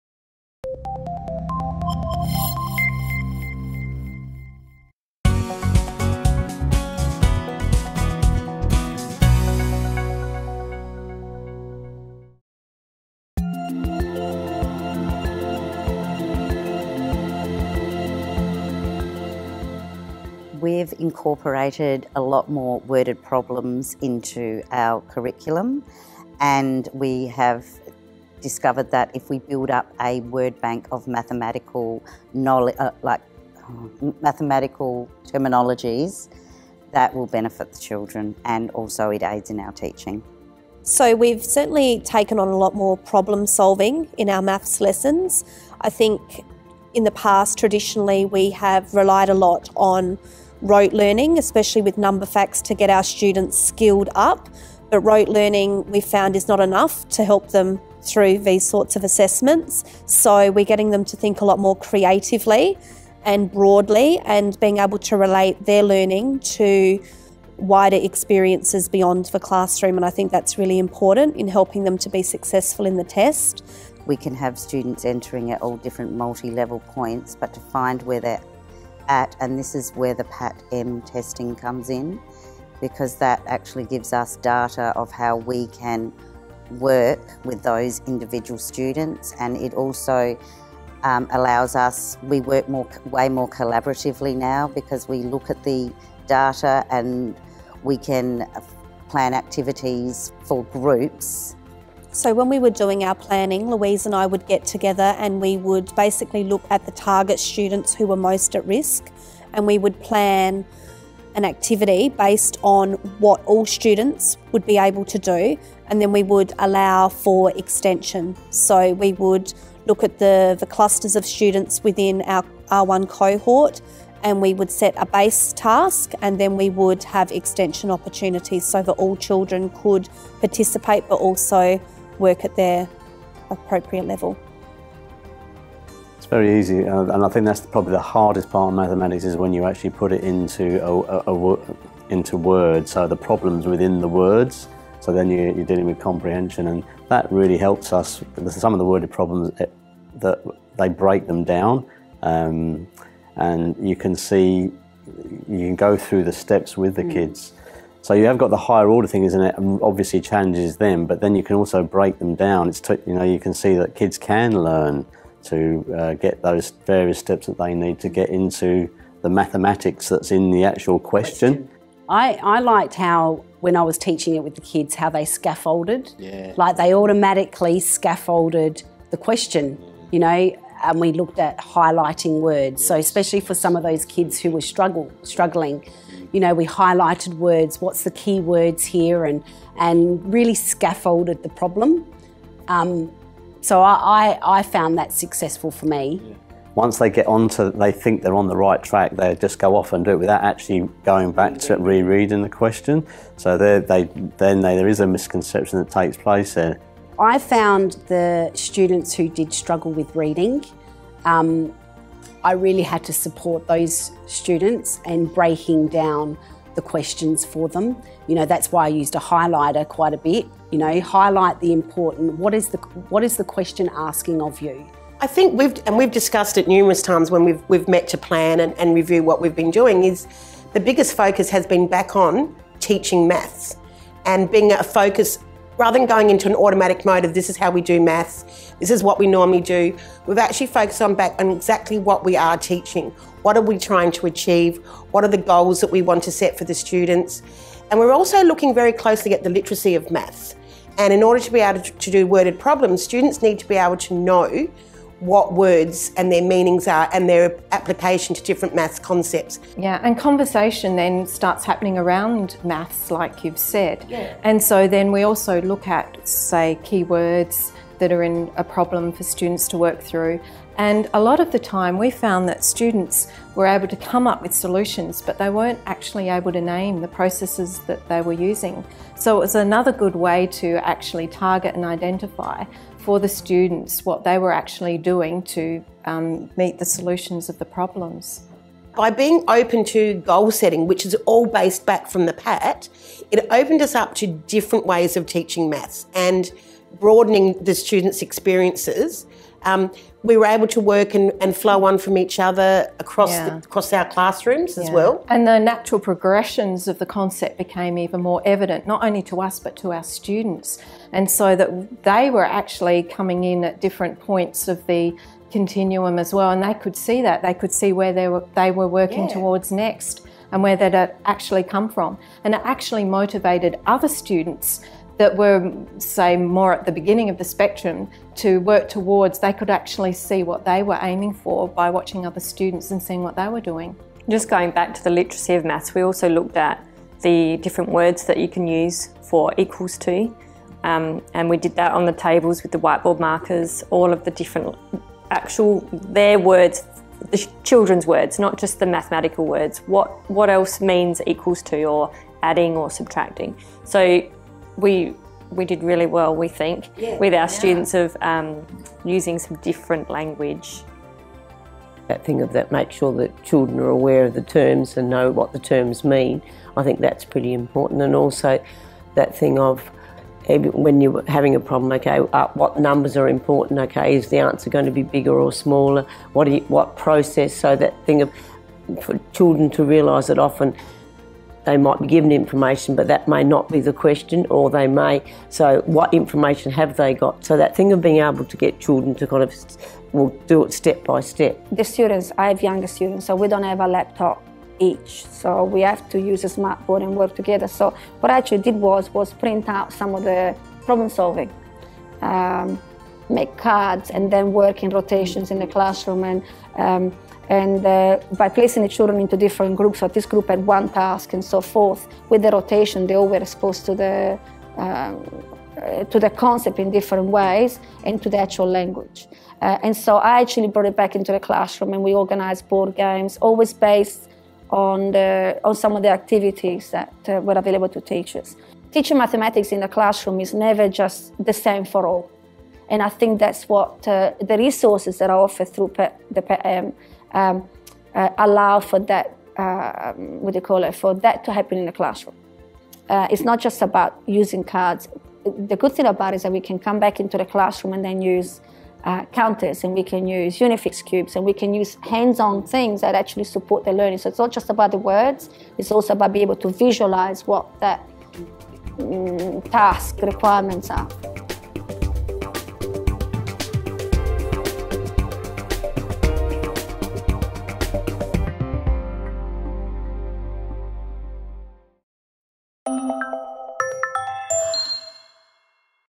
PAT Case Study: How PAT benefits students beyond the classroom
The ACER Progressive Achievement (PAT) approach encourages teachers to make changes to their practice in order to better meet student needs. In this interview, educators from Our Lady of the Visitation School in South Australia talk about incorporating problem-solving and worded questions into their early years maths curriculum - and the benefits this change has brought to their students well beyond the classroom.

More than half of all Australian schools use PAT. Not one of them?